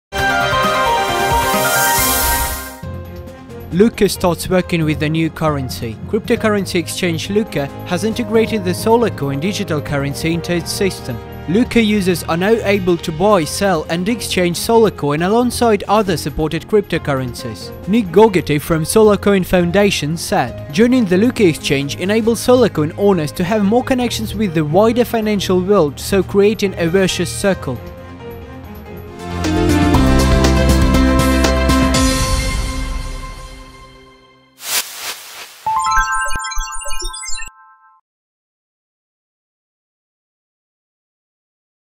KCN: SolarCoin integrates to Lykke Exchange
Lykke starts working with new currency. Cryptocurrency exchange Lykke has integrated the SolarCoin digital currency into it’s system. Lykke users are now able to buy, sell, and exchange SolarCoin alongside other supported cryptocurrencies. Nick Gogerty from SolarCoin Foundation said: “Joining the Lykke exchange enables SolarCoin owners to have more connections with the wider financial world, so creating a virtuous circle.”